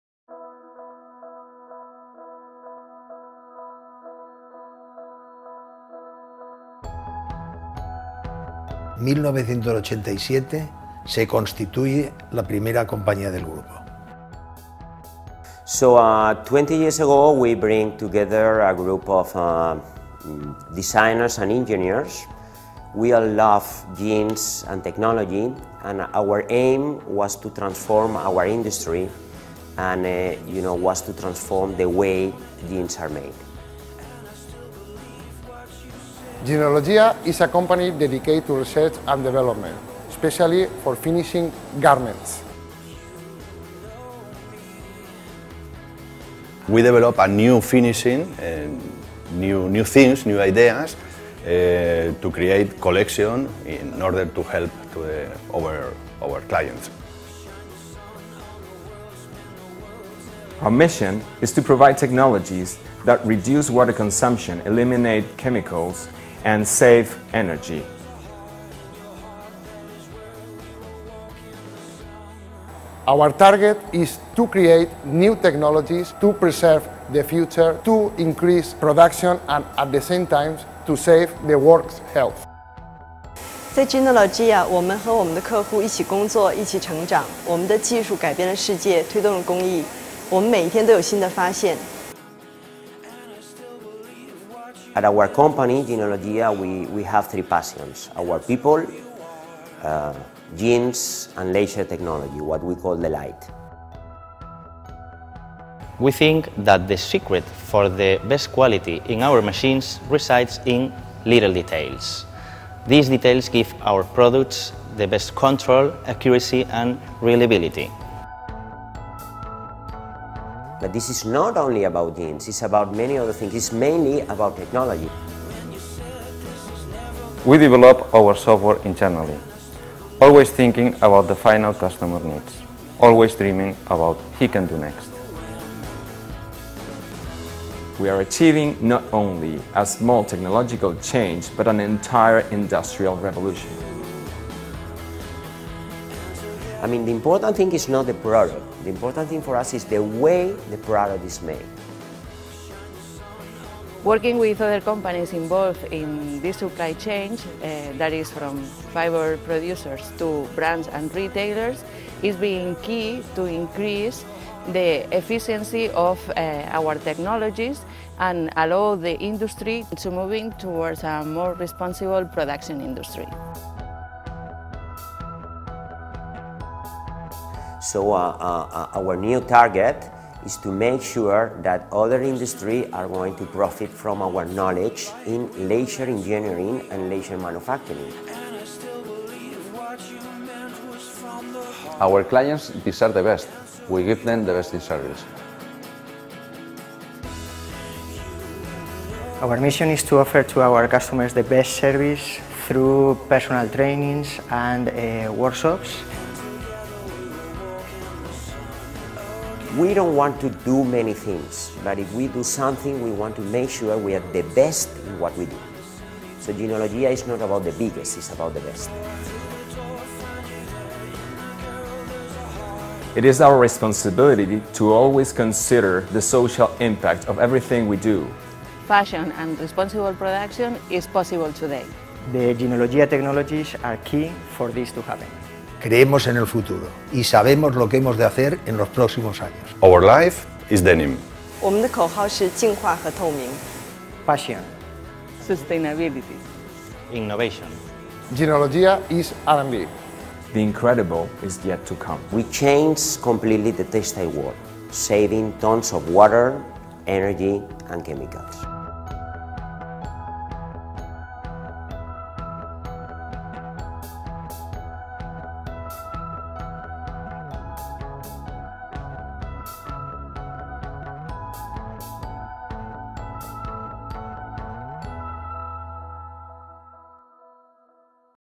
JEANOLOGIA: THE ORIGINS OF THE FORCE FOR GOOD (2010)
Today Jeanologia leads the transformation of the textile industry with its disruptive technologies (laser and eco systems) that enhance productivity, reduce water and energy consumption and eliminate damaging emissions and waste, guaranteeing ZERO contamination. We believe in business as a force for good.